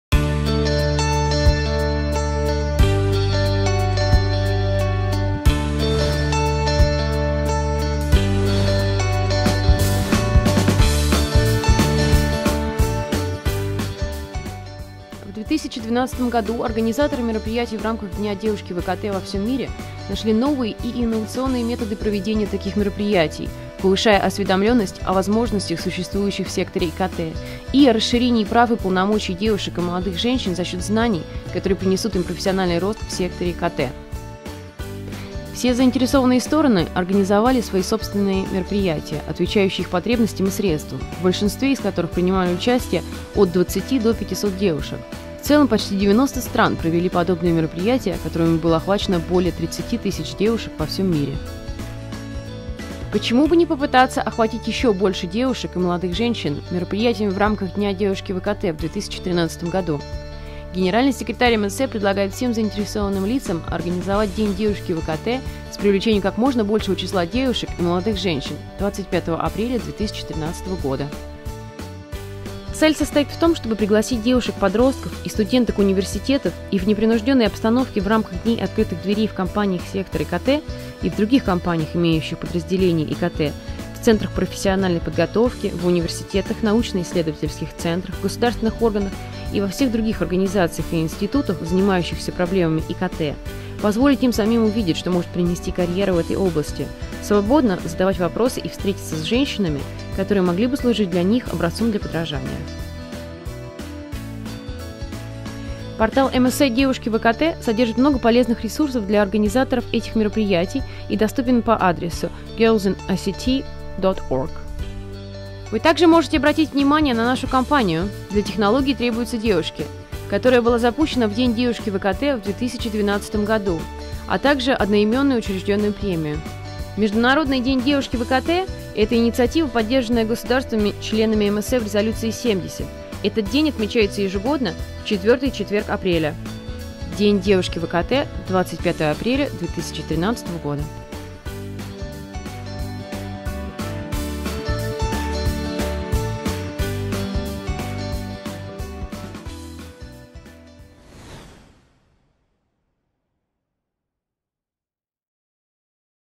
Дня "Девушки в ИКТ" в 2013 году
Russian version of the 2013 ITU video promoting Girls in ICT Day 2013, featuring video contributions from this year's Girls in ICT Day events around the globe.

What is Girls in ICT Day?
International Girls' in ICT Day is an initiative backed by ITU Member States in ITU Plenipotentiary Resolution 70 (Guadalajara, 2010) to create a global environment that empowers and encourages girls and young women to consider careers in the growing field of information and communication technologies (ICTs). International Girls in ICT Day is celebrated on the 4th Thursday in April every year.

International Girls in ICT day will next be observed on 25 April 2013.

What is a Girls in ICT Day event?  These are events where girls and university students are invited to spend the day at the office of ICT companies and government agencies so they better understand the opportunities the ICT sector holds for their future.  Ministries of ICT, Education, Labour, Youth, National ICT Regulatory Authorities, ICT companies, academic institutions, relevant UN agencies, NGOs and other stakeholders are encouraged to organize Girls in ICT day events on 25 April 2013. The ITU Secretary General invites all ITU Member States and Sector Members to organize events attracting as many girls and young women as possible.

To ensure that interested students can find more information about such events in their country, the ICT Studies and Careers Section includes links to national Girls in ICT Day activities.  Public and private sector organizations are encouraged to consult the Girls in ICT Day Toolkit to help plan a Girls in ICT Day event.  This Toolkit was developed by WITNET, the Global Network of Women ICT Decision Makers created in line with ITU Plenipotentiary Resolution 70.

Some film shots show impressions of the annual Girls' Day in Germany, funded by the Federal Ministry for Family Affairs and the Federal Ministry of Education and the European Social Funds, more